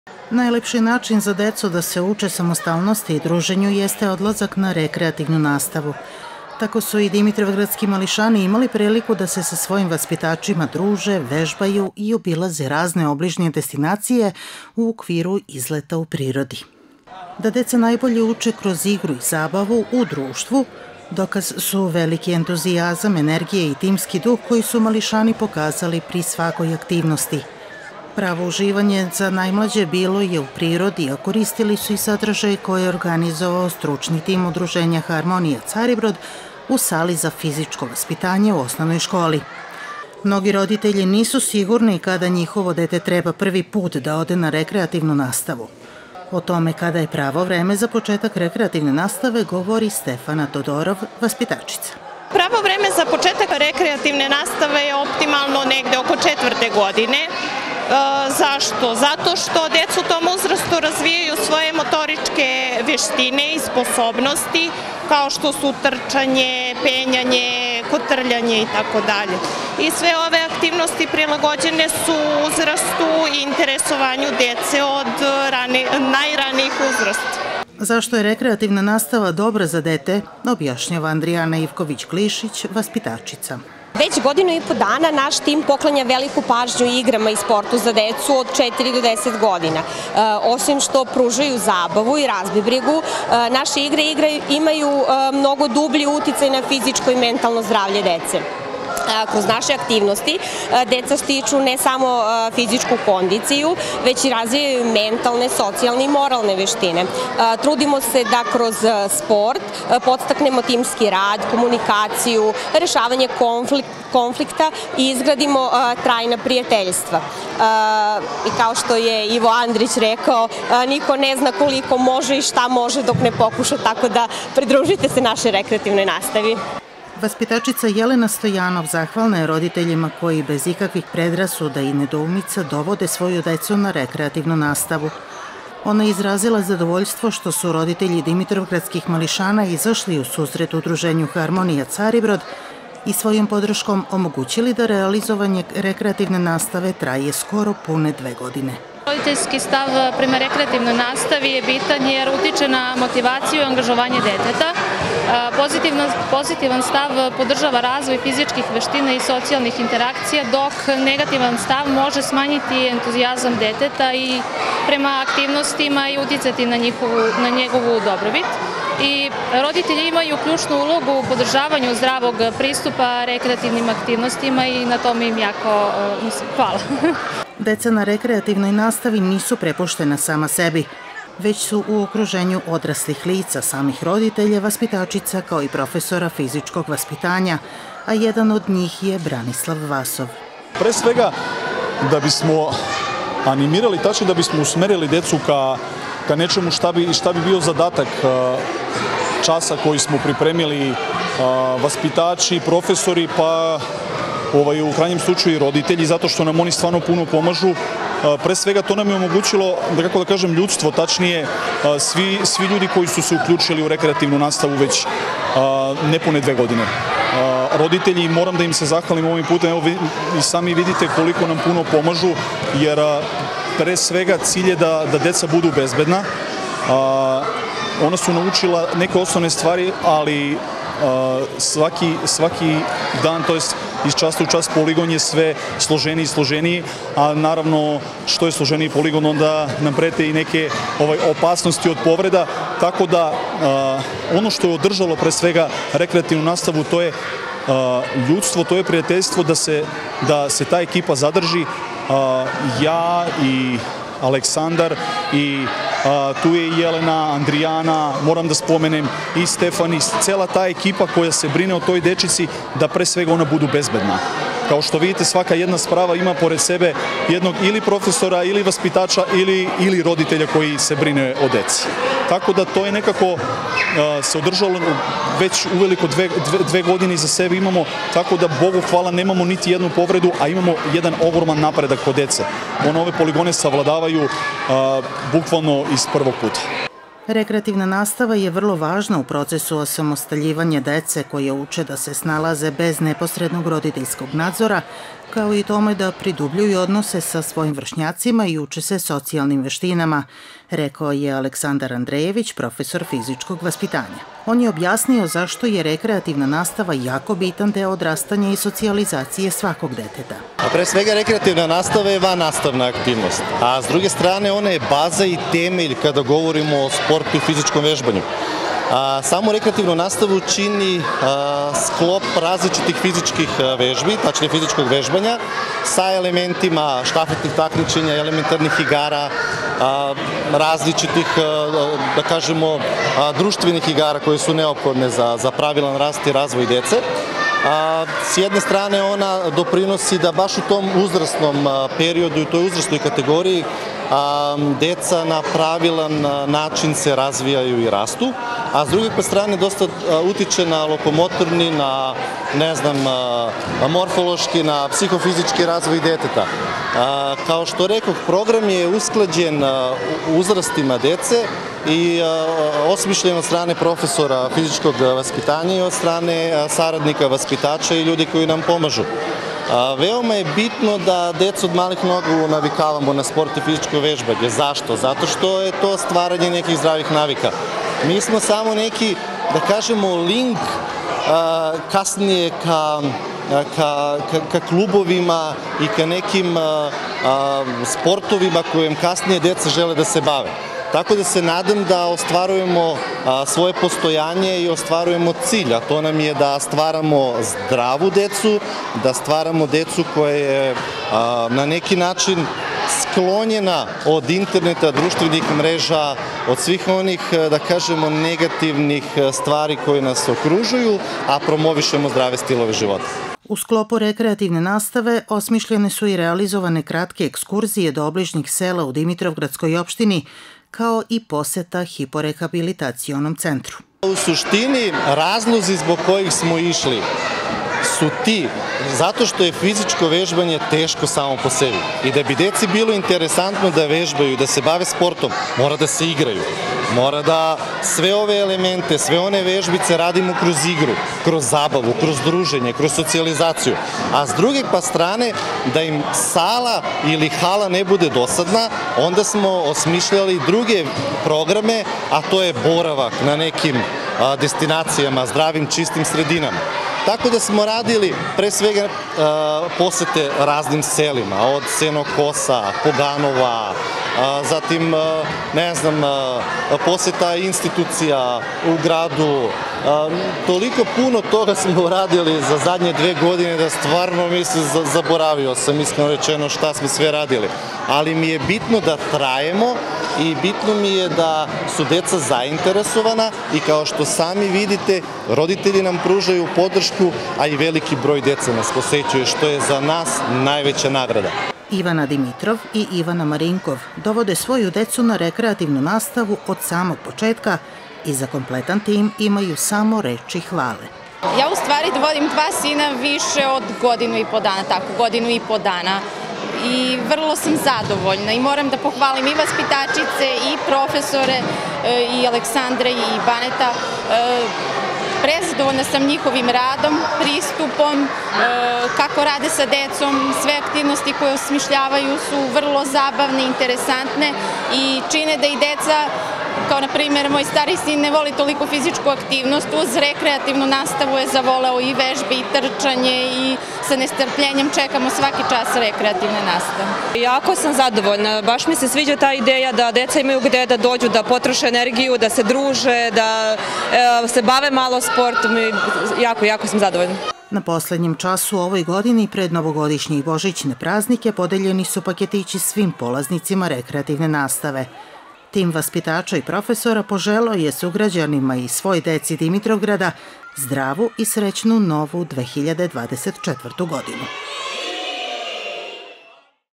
ORGANIZATORI REKREATIVNE NASTAVE KRAJ USPEŠNE GODINE OBELEŽILI SA DECOM UZ DODELU PAKETIĆA PRILOG
ORGANIZATORI REKREATIVNE NASTAVE KRAJ USPEŠNE GODINE OBELEŽILI SA DECOM UZ DODELU PAKETIĆA PRILOG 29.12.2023.